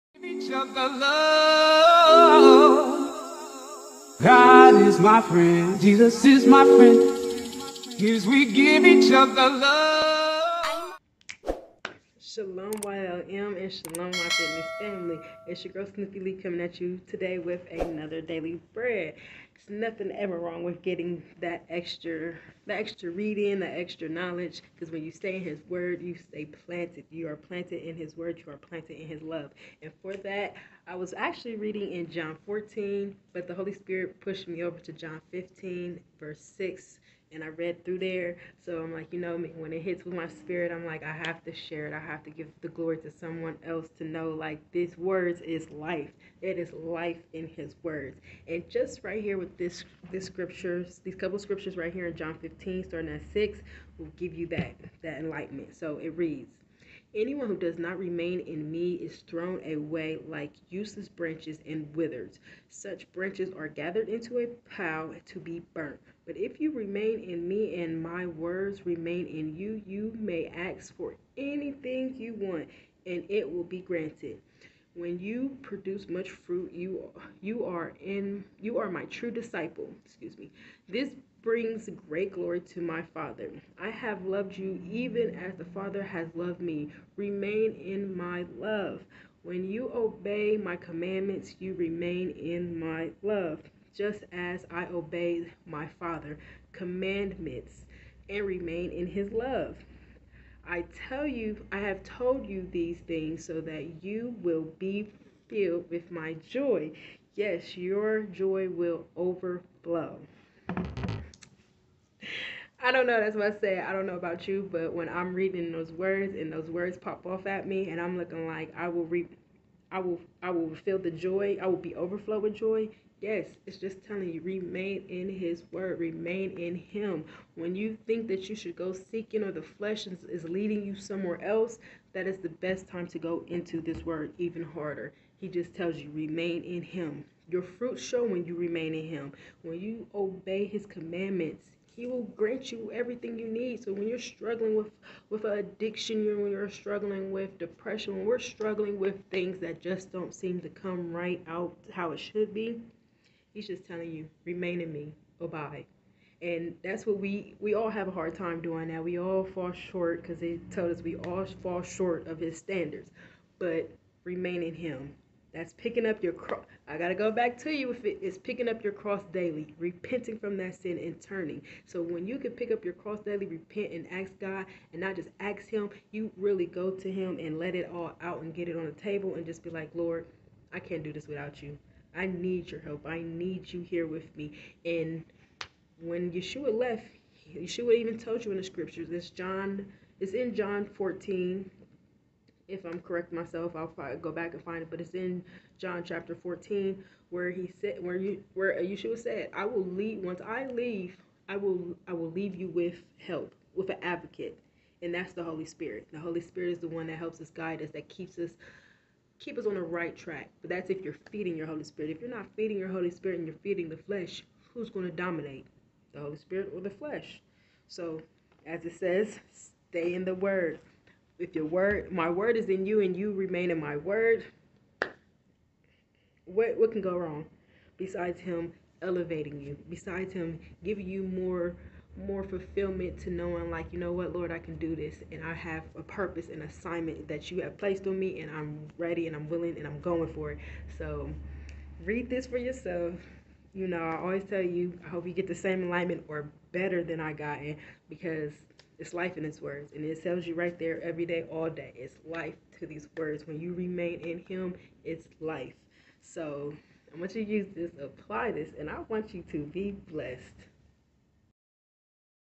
✨OUR DAILY BREAD ✨🍞 Come read with me in john15:6
Today's Word Came from john 15:6

😇 Shalom YLM Family 😇

 *Romans 15:13*
I pray that God, the source of hope, will fill you completely with joy and peace because you trust in him then you will overflow with confident hope through the power of the Holy Spirit.